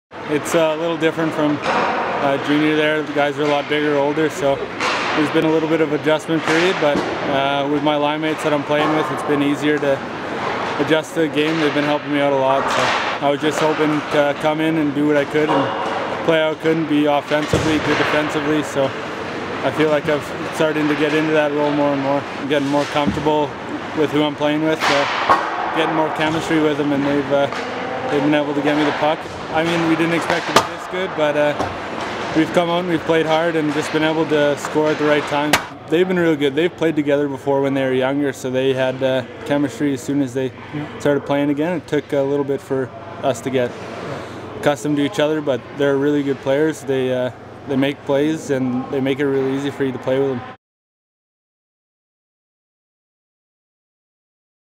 Rookie Levi Cable contributes to 9-1 start by Huskies hockey team
Playing on a line with fellow rookies Kohl Bauml and Andrew Johnson, Levi Cable has two goals and four assists for six points in 10 games as he becomes more comfortable with the CIS game. Cable played four seasons with the Western Hockey League’s Kootenay Ice, scoring 28 goals last season as a 20-year-old.